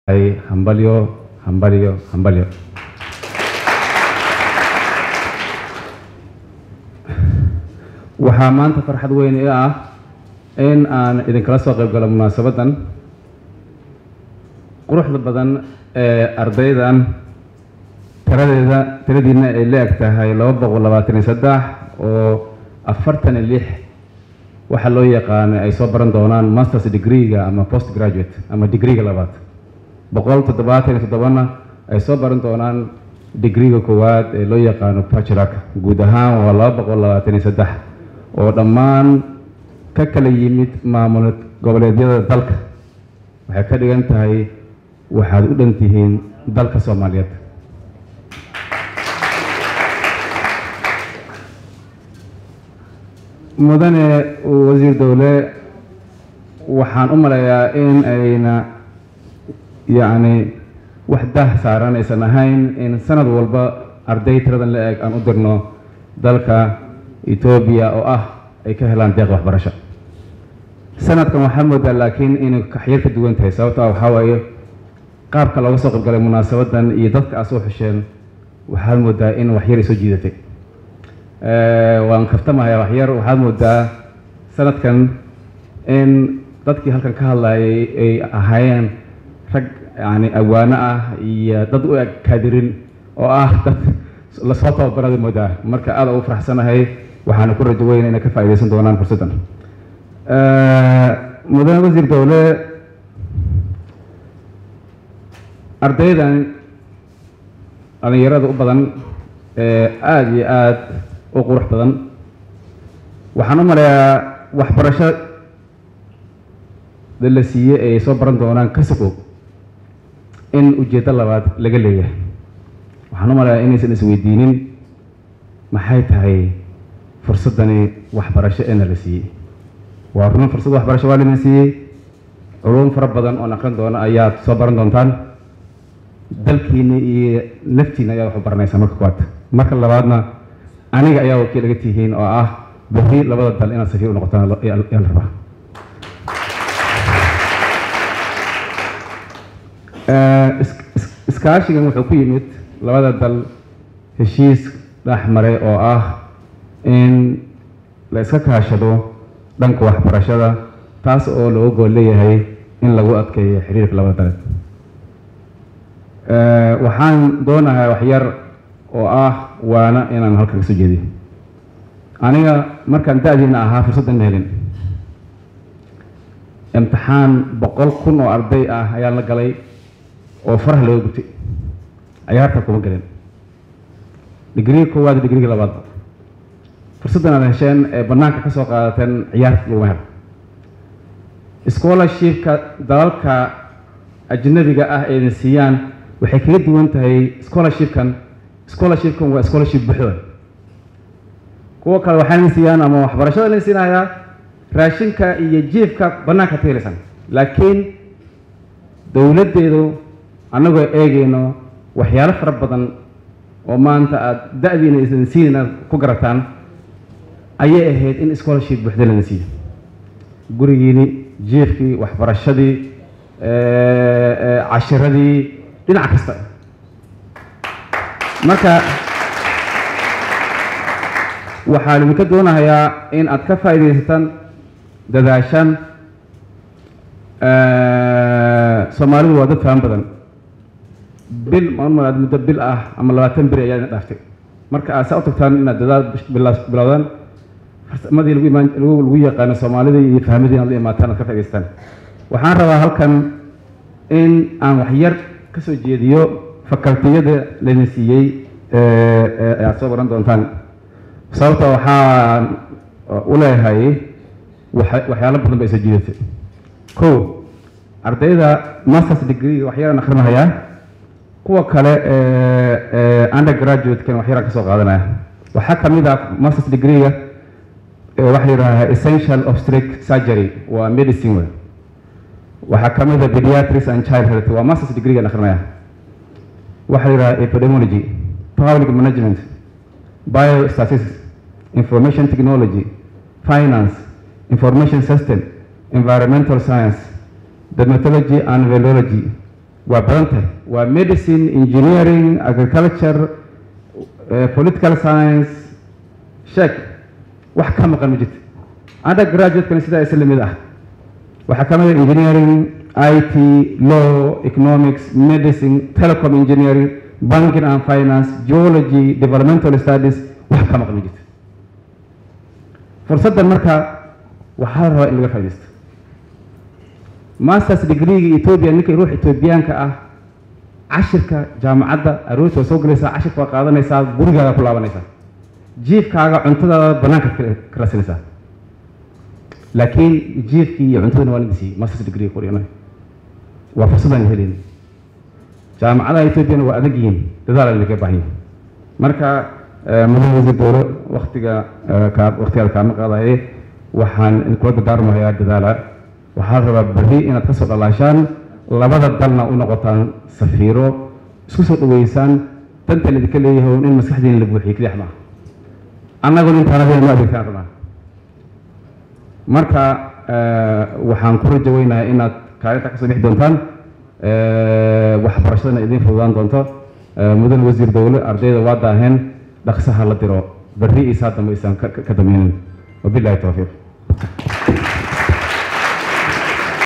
Safiirka Itoobiya u jooga Soomaaliya oo ka hadlay deeq waxbarasho
Safiirka Itoobiya u jooga Soomaaliya oo ka hadlay deeq waxbarasho oo ay siiyeen dhalinyaro Soomaaliyeed